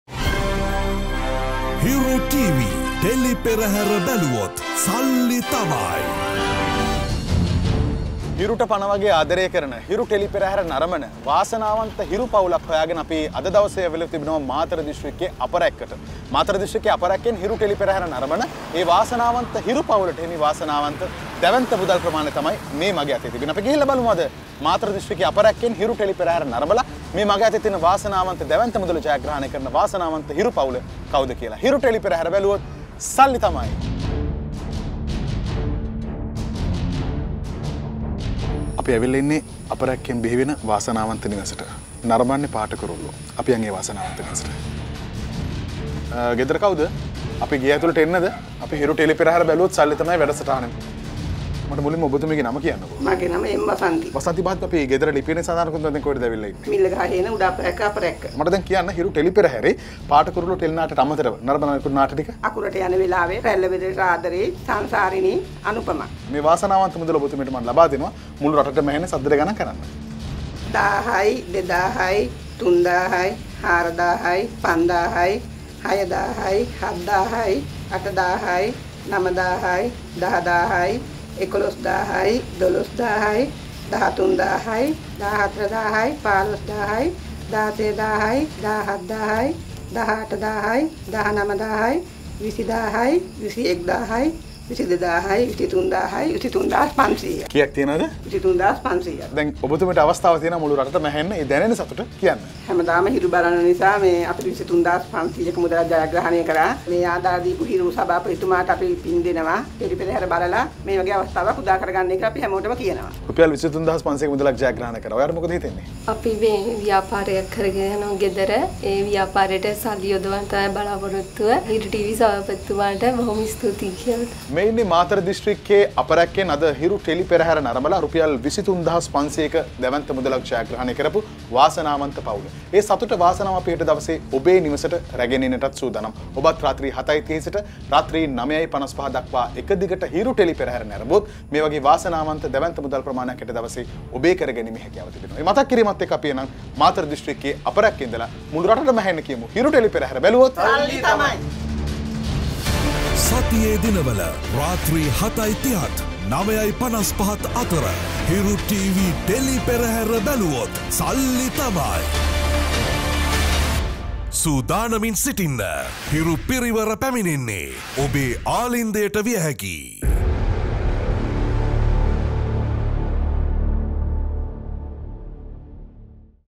"හිරු ටෙලි පෙරහැර බැලුවොත් සල්ලි තමයි"
හිරු ටෙලි පෙරහැර නරඹා මාතර  දිස්ත්‍රික්කයේ, අපරැක්ක  ප්‍රදේශයෙන් රුපියල් 23,500 ක මුදලක් දිනාගත් එම්. වසන්ති